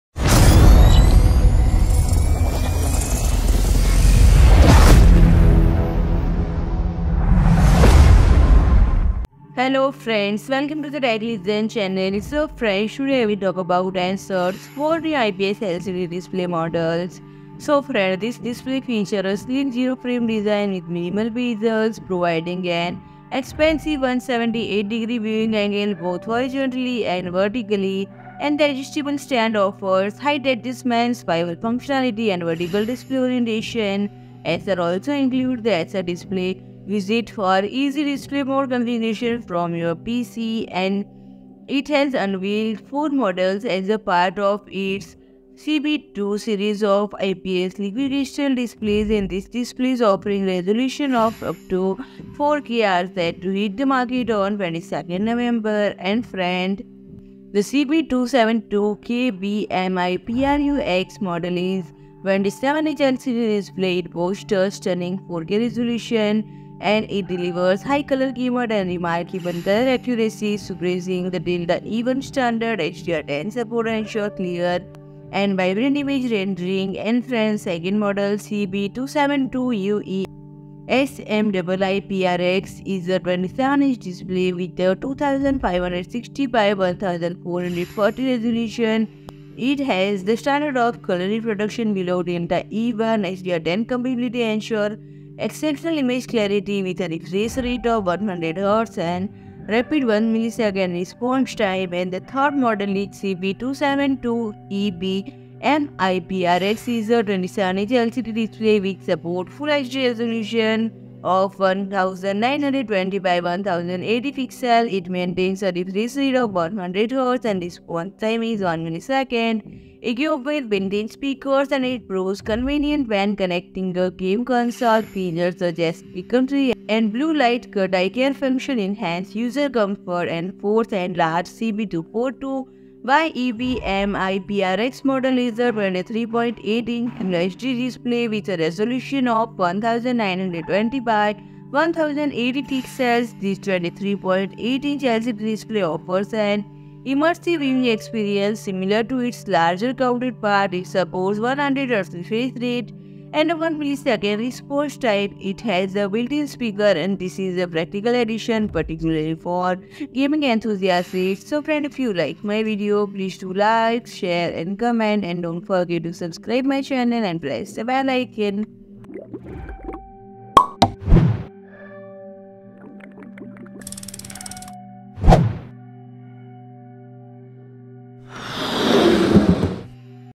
Acer Four New IPS LCD Display Models Including 27-Inch 4K Display Launched - Explained All Details
PRODUCT BRAND STORE AMAZON

CB272Kbmiprux (27-inch 4K Display):

This 27-inch LCD display boasts a stunning 4K resolution of 3,840 x 2,160.
It delivers high color gamut and remarkable color accuracy, surpassing the Delta E1 standard.
HDR10 support ensures clear and vibrant image rendering.
It features USB Type-C input and provides a 90W USB PD power supply, enabling seamless connectivity with a single cable to your notebook PC.
CB272UEsmiiprx (27-inch Display with 2,560 x 1,440 Resolution):

Offering a resolution of 2,560 x 1,440 pixels, this 27-inch liquid crystal display upholds the standard of color reproduction below Delta E1.
HDR10 compatibility ensures exceptional image clarity.
With a refresh rate of 100Hz and a rapid 1ms response time, it's ideal for smooth visuals.
CB272Ebmiprx (27-inch Full HD Display):

This 27-inch LCD display supports full HD resolution at 1,920 x 1,080 pixels.
It maintains a refresh rate of 100Hz and a responsive 1ms.
Equipped with built-in speakers, it proves convenient when connecting a game console.
Features such as flicker-free and blue light-cut eye care functions enhance user comfort.
CB242YEbmiprx (23.8-inch Full HD Display):

With a full HD resolution of 1,920 x 1,080 pixels, this 23.8-inch LCD display offers an immersive viewing experience.
Similar to its larger counterpart, it supports a 100Hz refresh rate and a 1ms response time.
The built-in speaker is a practical addition, particularly for gaming enthusiasts.